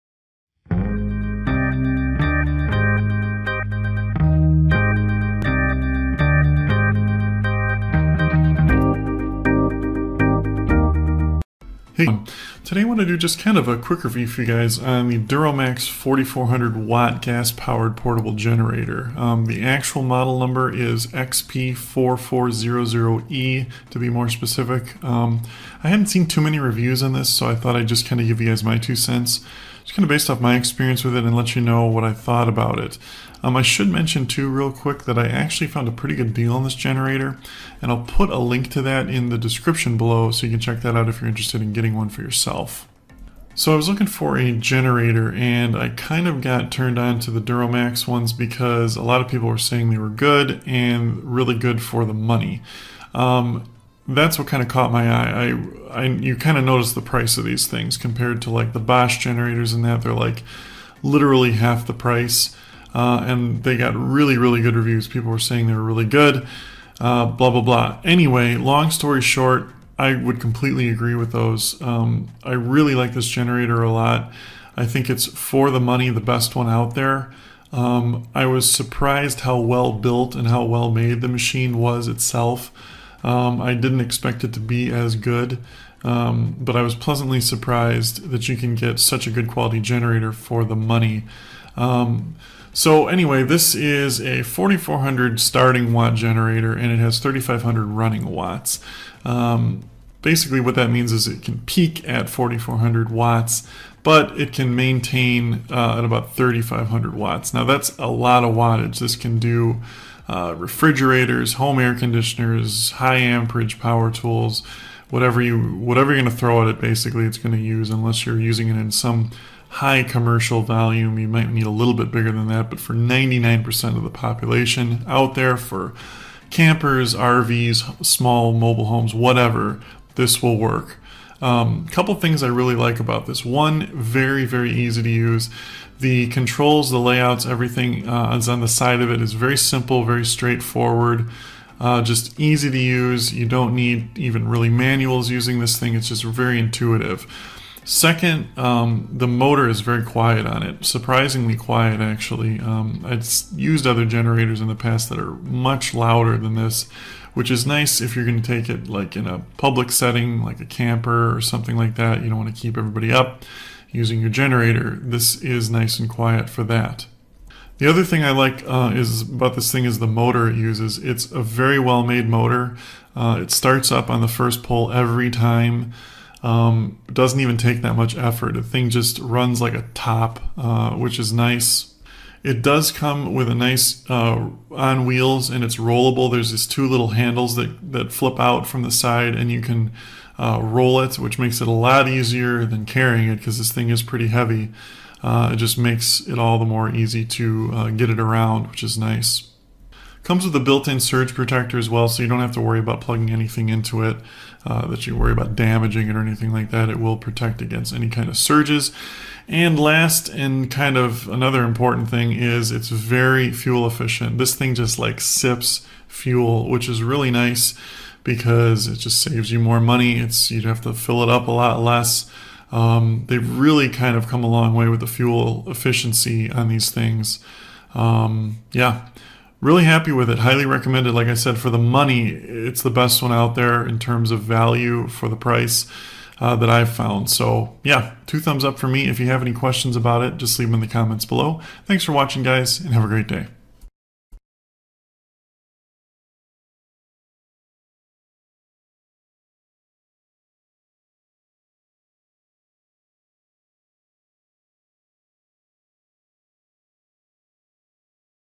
DuroMax XP4400E 4400 Watt Portable Generator - Review 2022
DuroMax XP4400E Gas Powered Portable Generator-4400 Watt Electric Start-Camping & RV Ready, 50 State Approved, Blue/Black
+ Plenty of Power – With 4,400 starting watts and 3,500 running watts, this unit can handle heavy loads, from lights and a refrigerator to a home air conditioner and high amperage power tools
+ Powerful Engine – The DuroMax 210cc OHV engine is a workhorse that provides plenty of power to handle multiple jobs, from powering high voltage appliances to heavy duty power tools
+ MX2 Technology - Get the maximum power from each of the 120-volt receptacles. Choose between operating the generator at both 120V and 240V simultaneously, or at 120V only with full power. Ideal for usage with RVs
+ Low Oil Shutoff – Protects your investment by automatically shutting the generator off when it senses that the oil is low
+ Fully Featured Power Panel – The power panel includes a wide selection of outlets for maximum compatibility in your applications including: 2 120V household GFCI outlets and 1 120/240V 30A twist lock outlet. The panel also includes a voltmeter and 12V DC charging posts for charging external batteries

@ B001BMDFPS